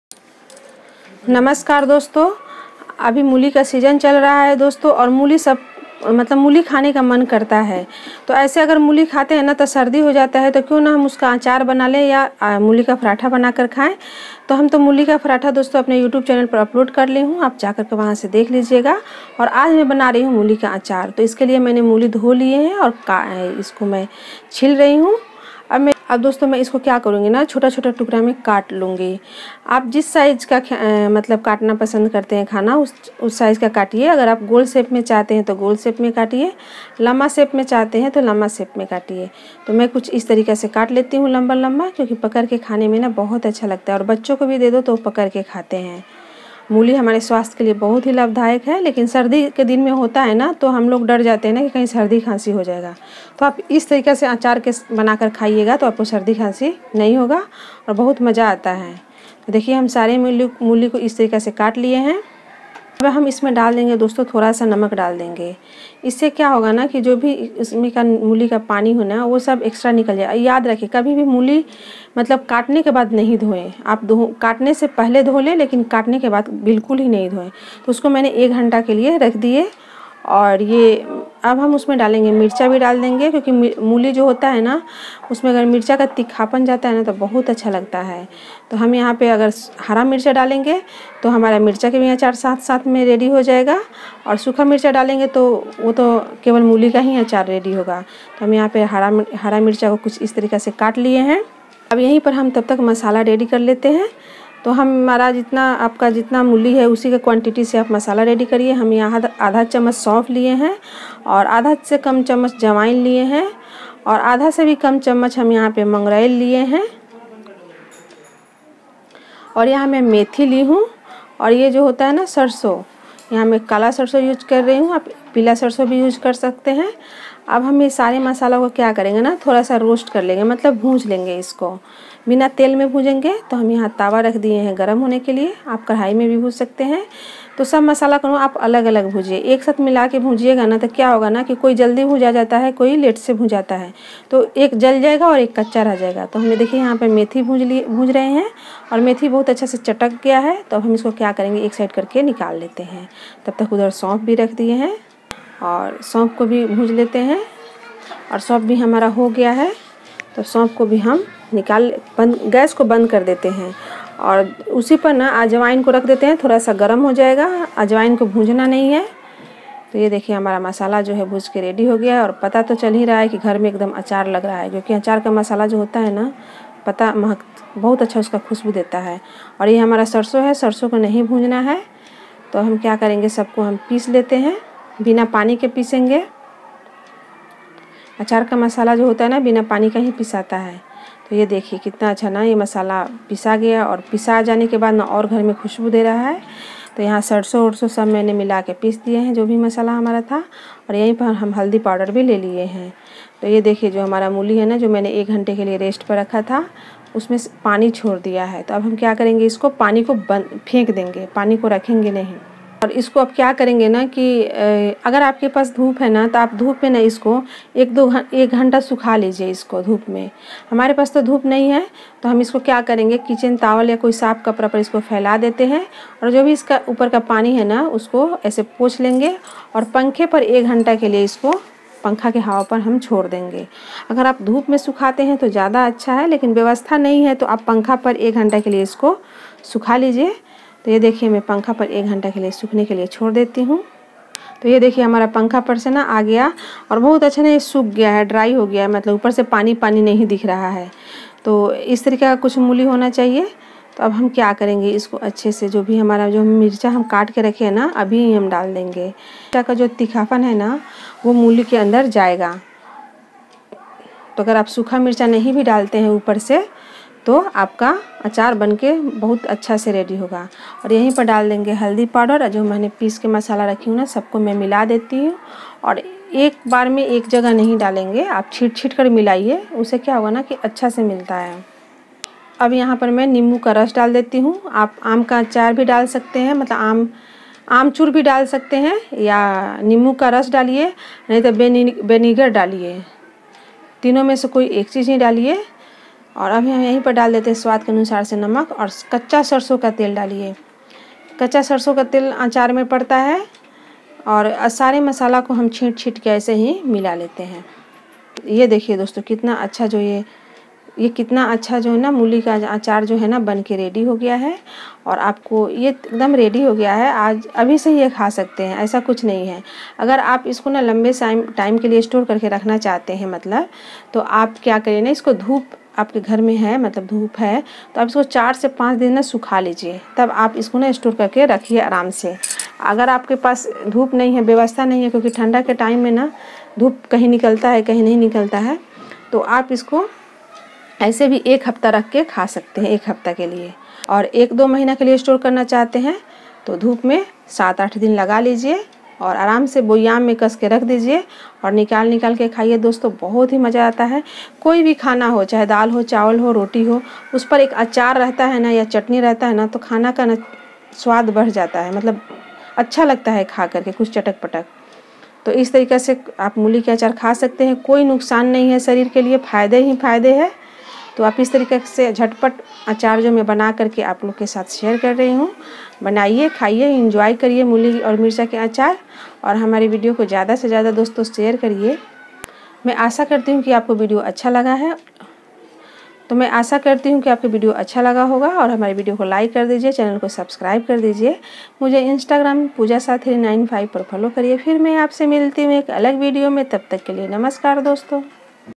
मूली और मिर्च का आचार, जो झटपट बने और बनते ही खाया जाए। मिर्च और मूली का आचार recipe।
मिर्च और मूली का instant achar
reddish pickle
muli recipe
radiesh ki recipe
achar ki recipe aam ka achar
mirch ka achar puja food vlog
pari ki duniya